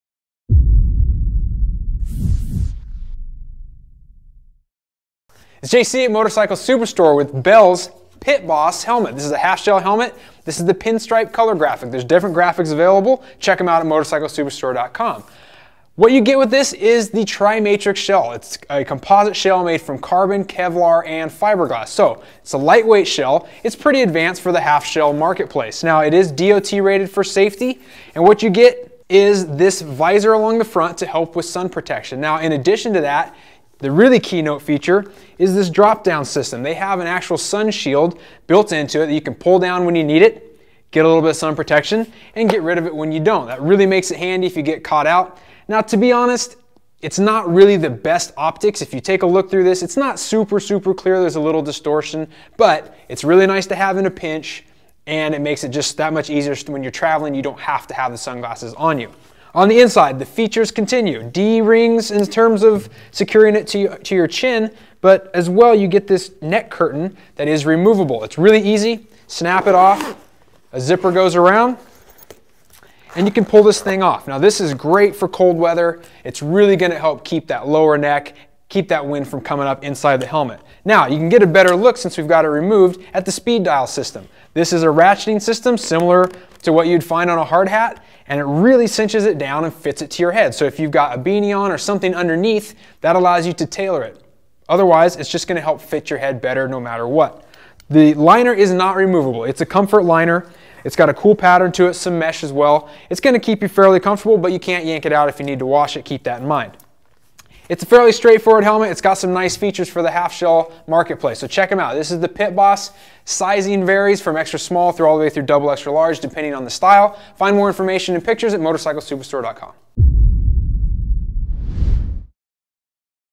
Bell Pit Boss Pinstripe Helmet from Motorcycle-Superstore.com
From the first time you put the Pit Boss helmet on, you can feel the difference. The ultra-light TriMatrix construction keeps weight to a minimum while still passing tough DOT standards. The Pit Boss also features Bell's innovative Speed Dial adjustable fit system which provides unmatched levels of comfort while drastically reducing the tendency for open face helmets to lift off of the head. And with additional features like a removable neck curtain and drop down sunshield, the Pit Boss truly reigns supreme on the wide open road.

Features:
Lightweight, TriMatrix composite shell—Kevlar/Carbon/Fiberglass blend
Bell's innovative new Speed Dial adjustable fit system is adjustable both vertically and horizontally, drastically reducing the tendency for open face helmets to lift off of the head
Internal Sun Shade offers "on the fly" protection from the sun
Removable neck curtain for cold weather riding.
Convenient speaker pockets make it easy to set up the Pit Boss with communication devices.
Five-year warranty
DOT certified